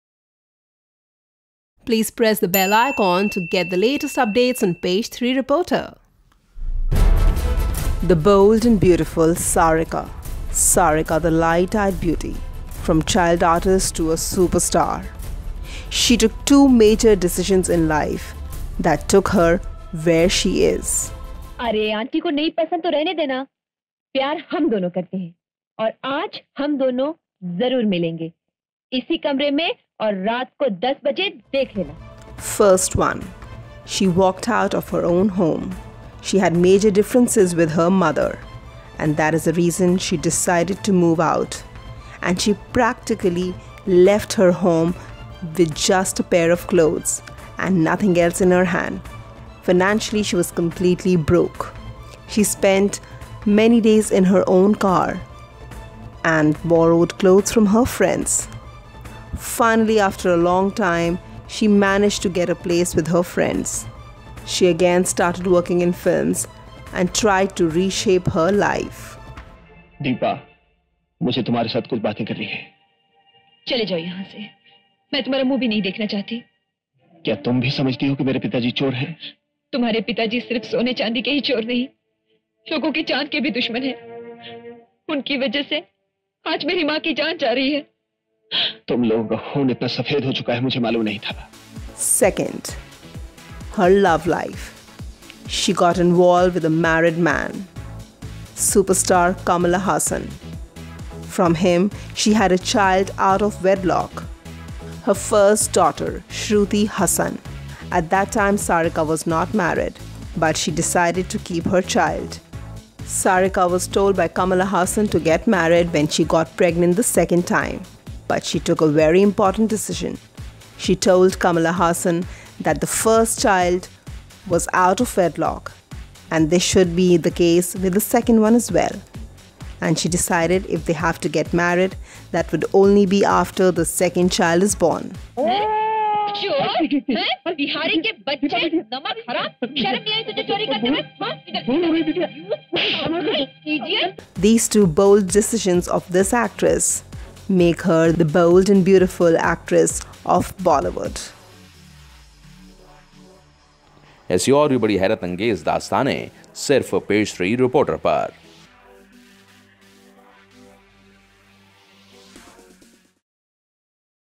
Sarika - The Bold And Beautiful Diva Of Bollywood
Sarika Thakur, better known as Sarika, is an Indian actress. Apart from acting, she also served as costume designer, sound designer & associate director for Kuruthipunal under Raaj Kamal Films International. In 2005, she won the National Film Award for Best Actress for the English film Parzania.